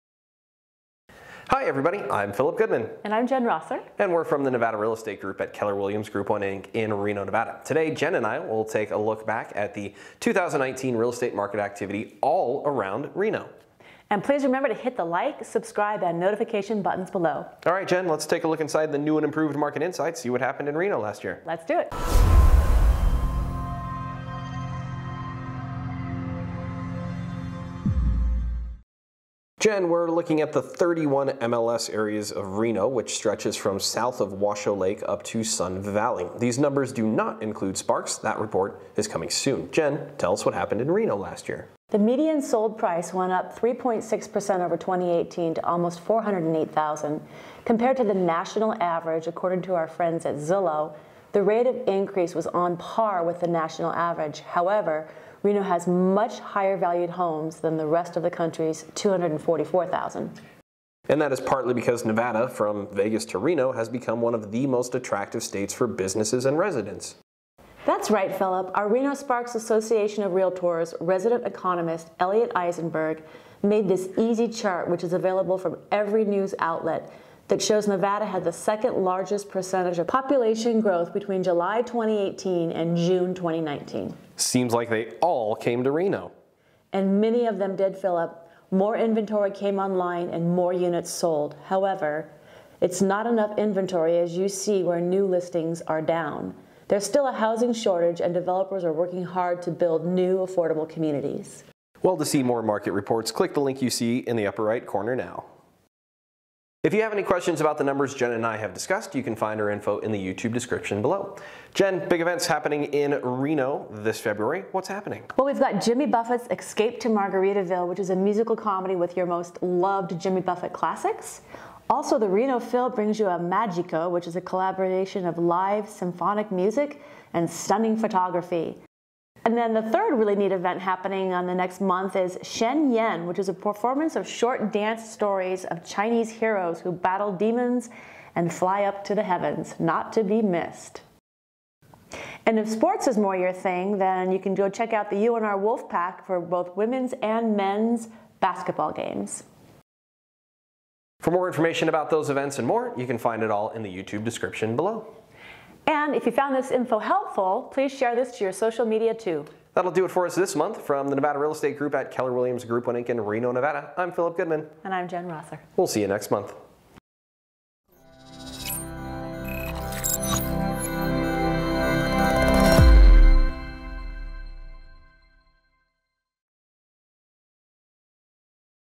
Reno, NV Real Estate Market Report 2019 Year in Review | Nevada Homes for Sale
Jennifer Rosser and Phillip Goodman review the real estate activity in all of Reno, NV for the entirety of 2019.

For more info on events at The Pioneer.

For more info on events at the Reno Philharmonic,